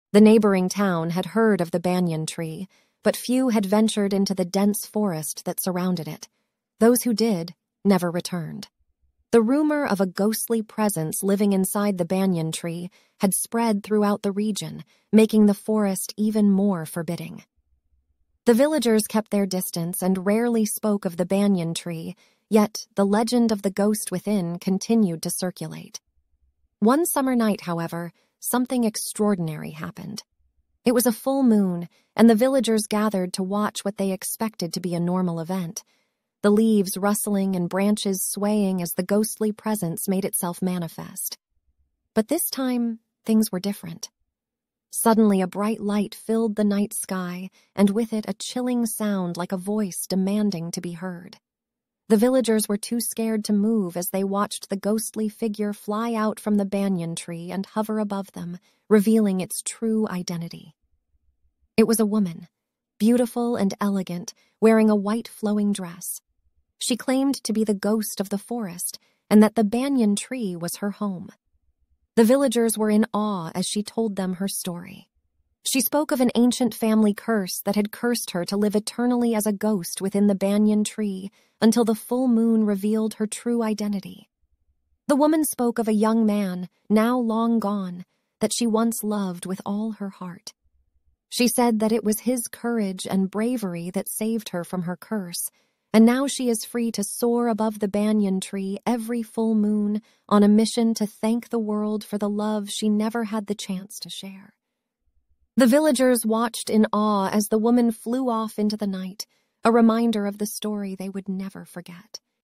Discover 'The Ghostly Banyan Tree': The First AI-Generated Kids' Story!
Prepare for an extraordinary storytelling experience like no other as we present 'The Ghostly Banyan Tree,' the first-ever children's story created entirely by Artificial Intelligence (AI). Immerse yourself in this enchanting video and let the AI take you on a thrilling journey through a world of mystery and wonder.

In 'The Ghostly Banyan Tree,' young readers will embark on a captivating adventure, where they will encounter the eerie secrets and haunting legends that surround the ancient banyan tree. Watch as AI's algorithms craft a spellbinding narrative that combines elements of magic, courage, and friendship.

This groundbreaking project showcases the remarkable capabilities of AI in the realm of storytelling. Witness how the algorithm analyzes vast amounts of data, explores patterns, and creates a unique story that will captivate and ignite the imagination of children.

Whether you're a parent seeking an enchanting storytime experience or an AI enthusiast fascinated by the potential of machine-generated narratives, 'The Ghostly Banyan Tree' is a must-watch. It represents a fusion of technology and creativity, delivering a tale that will leave you spellbound.

Join us as we delve into the future of storytelling, where AI's algorithms work hand in hand with human imagination to create magical narratives. Be a part of this extraordinary moment in children's literature and experience the wonders of 'The Ghostly Banyan Tree' brought to life by AI